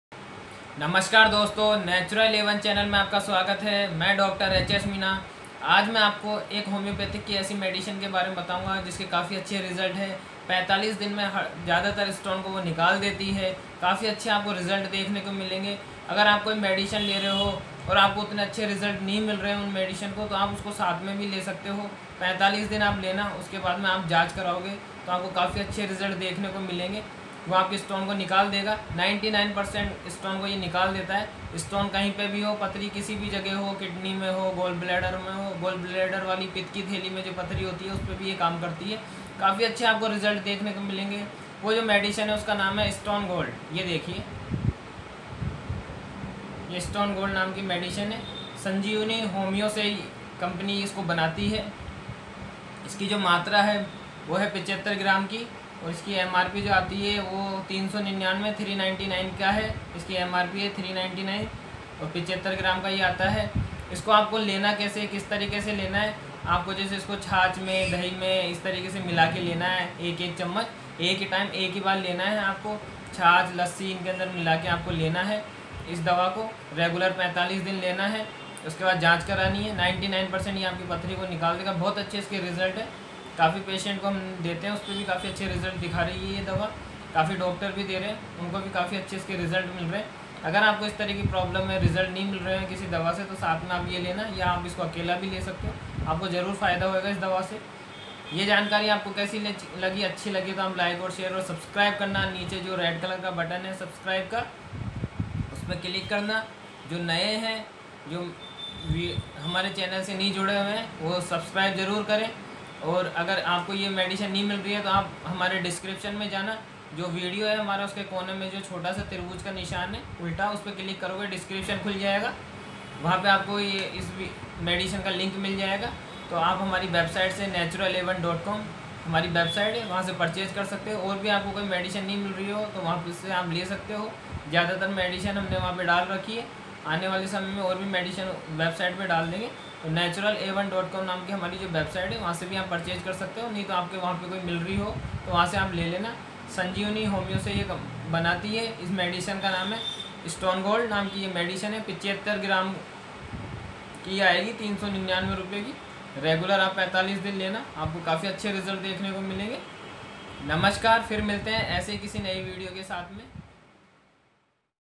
45 - Dino Mein Pathri (Stone) ko Bahar nikale , STONE GOLD Sanjeevani Homoeose medicine
45 दिनों में पथरी को भार निकालने वाली औषधि ।

होम्योपैथिक-आयुर्वेदिक औषधि खरीदे के लिये इस लिंक पर क्लिक करे।